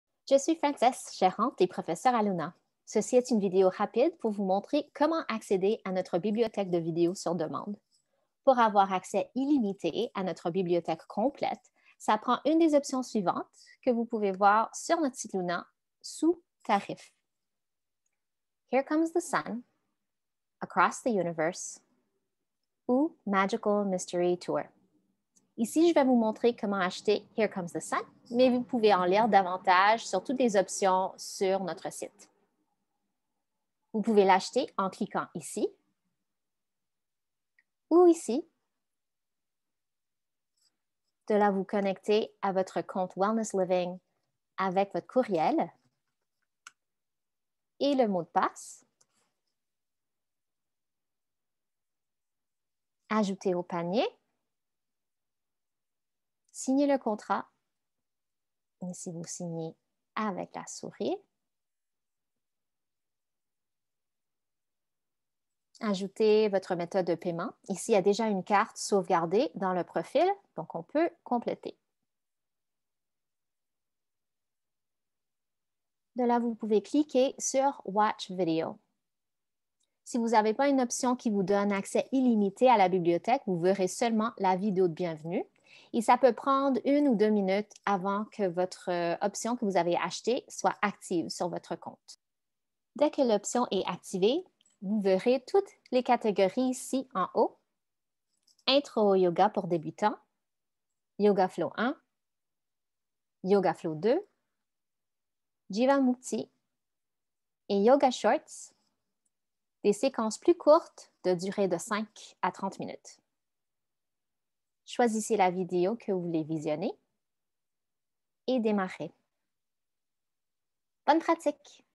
Comment accéder à notre bibliothèque de vidéos sur demande
Des instructions étape par étape pour vous montrer comment accéder à notre bibliothèque de vidéos sur demande.